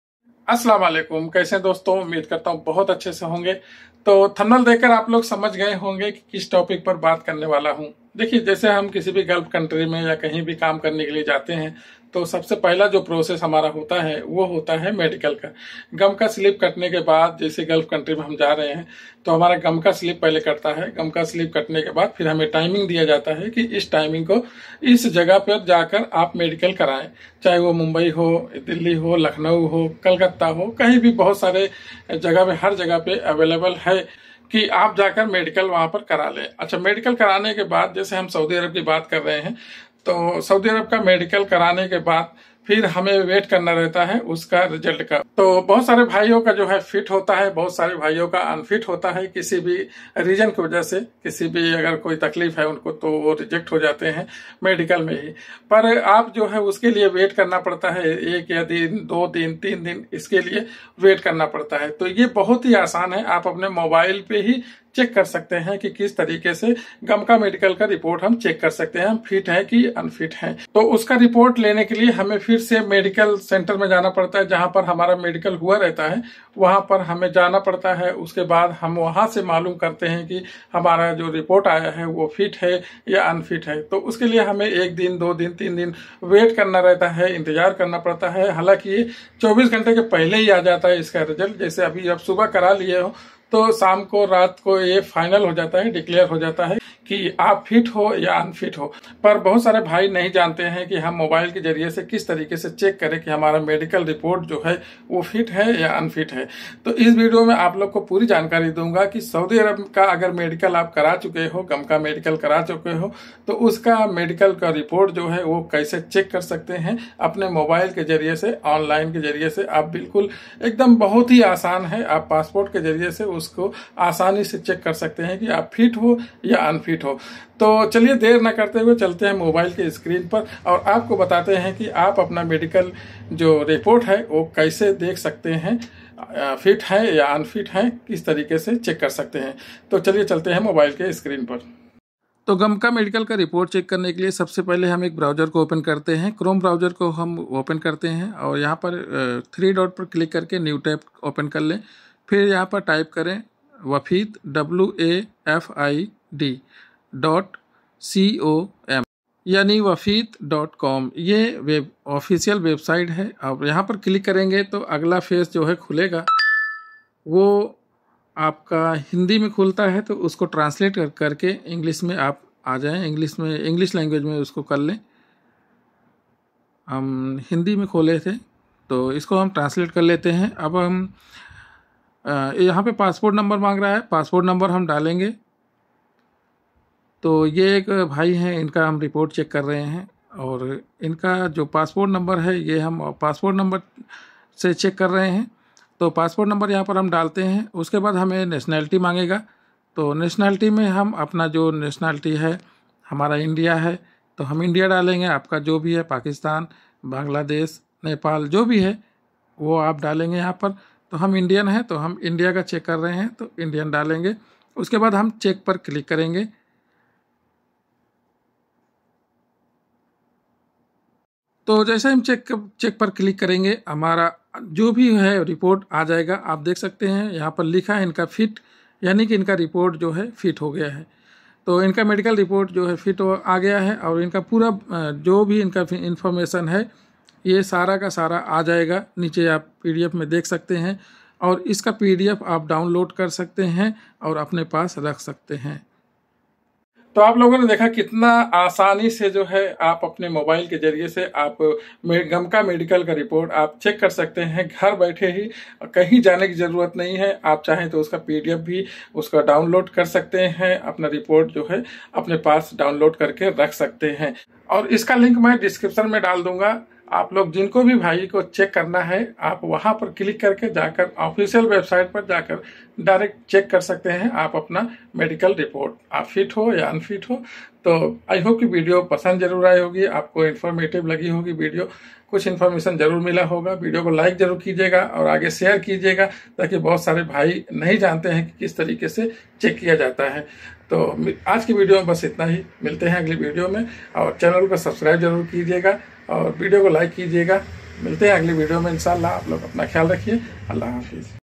गमका मेडिकल रिपोर्ट कैसे चेक करें l How To Check Gamca medical Report onlin#gamcareport#saudia#india
गमका मेडिकल रिपोर्ट कैसे चेक करें ll How To Check Gamca medical Report ll

Indian passport number se gamca medical report check Karen
Online check gamca medical report
Gamca medical report online kaise check kare
How to check gamca medical report by passport number

Gamca medical report check online

Wafid.com